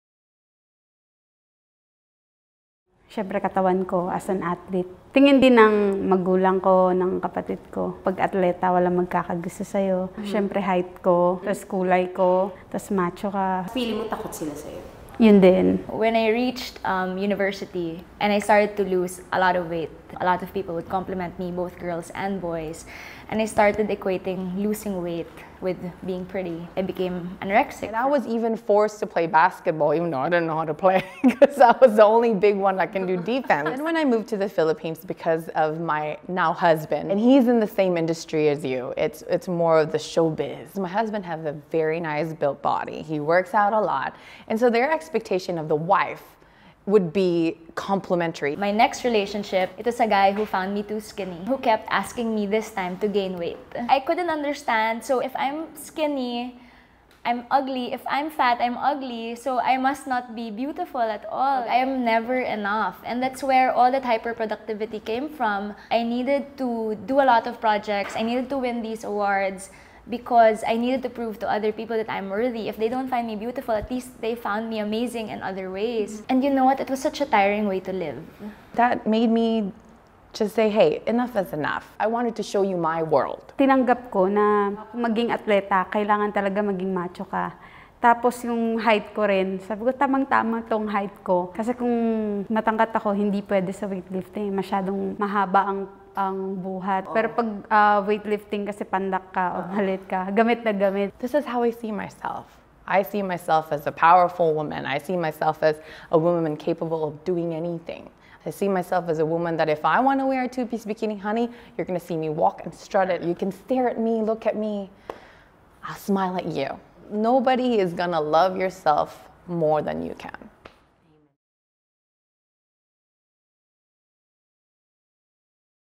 On What Others Think | The Body Love Revolution Stories
What do other people say about you and your body? And what was the effect of that on you? Three inspiring women, athlete Hidilyn Diaz, body positivity activist Rona Tai, and She Talks Asia co-founder Lynn Pinugu share their powerful stories.